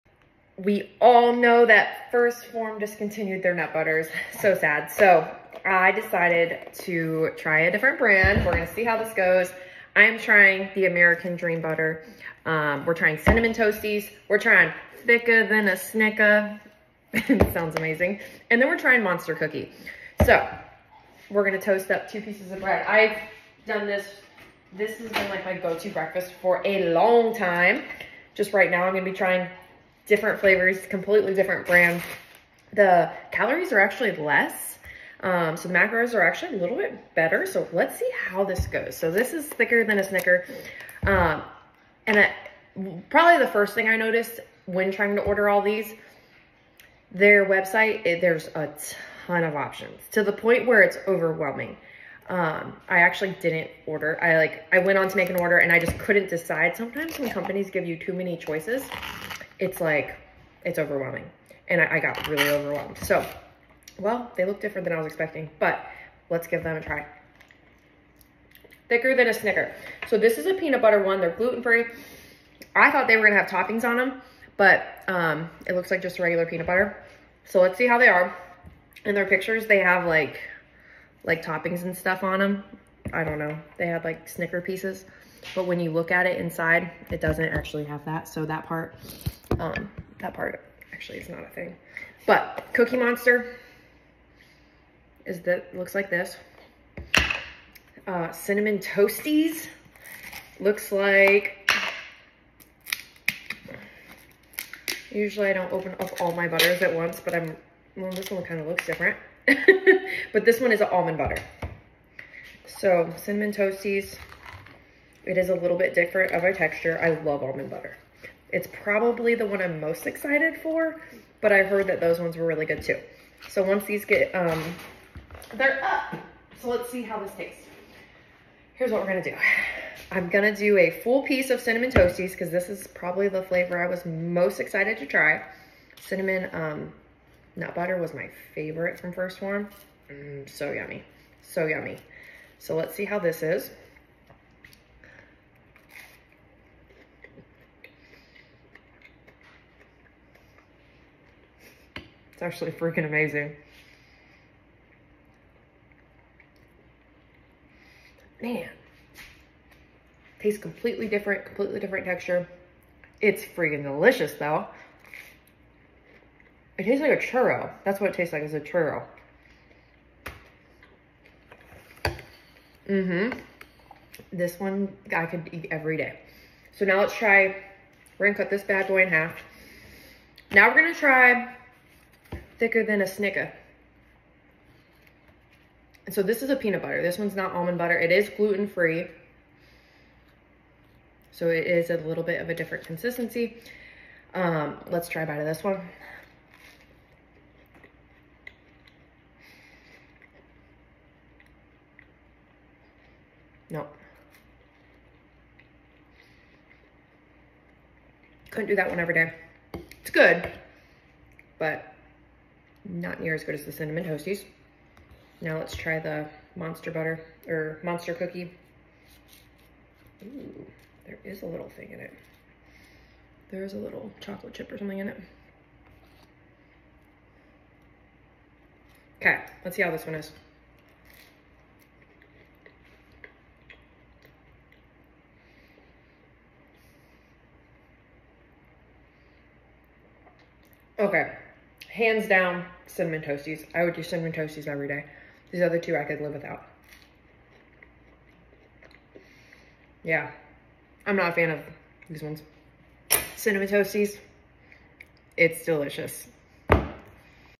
Let’s see which NUT butter we like best!
Let’s try out some of the American Dream Nut butters and see which one is the best!!

Here’s the website to find all these yummy flavors!!

What is your favorite flavor?